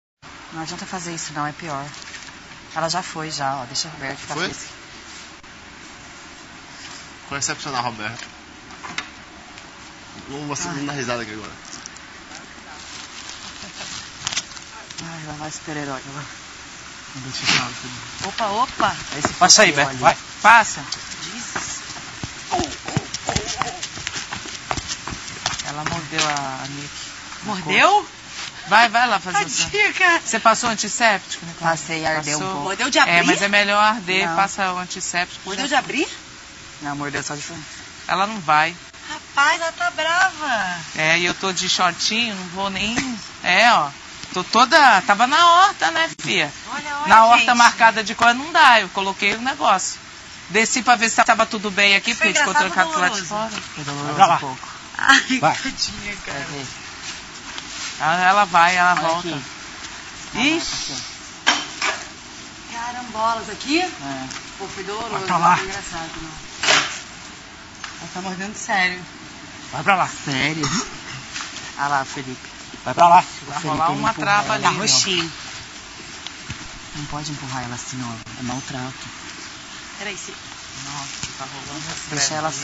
felipe folgosi e a lhama
Felipe Folgosi trata de forma incorreta a lhama e isso leva a que os peões levem uma bronca